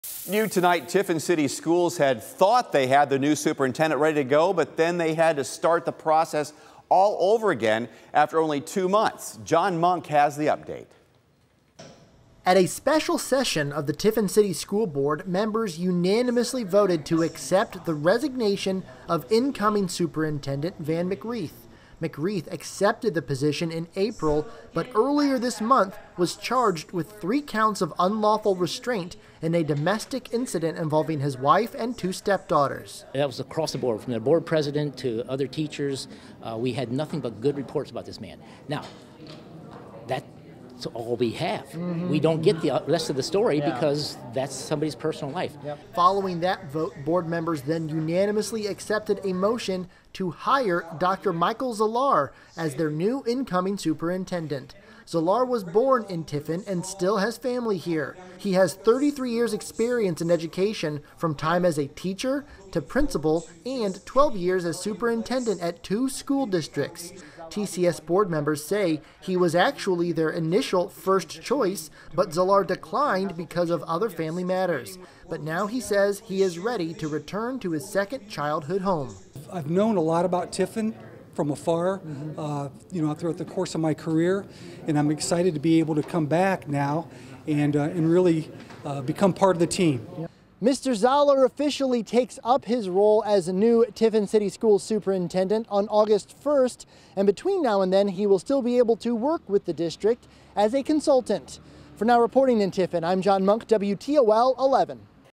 Tiffin school board accepts resignation of incoming superintendent McWreath; approves alternate hire
Tiffin City Schools had thought they had their new superintendent ready to go, but then had to start the process over again after only two months.

At a special session of the Tiffin City School Board, members unanimously voted to accept the resignation of incoming superintendent Van McWreath.

McWreath accepted the position in April, but earlier this month was charged with three counts of unlawful restraint in a domestic incident involving his wife and two stepdaughters.

Dr. Michael Zalar was initially the board's first choice but now accepts position following McWreath domestic issues.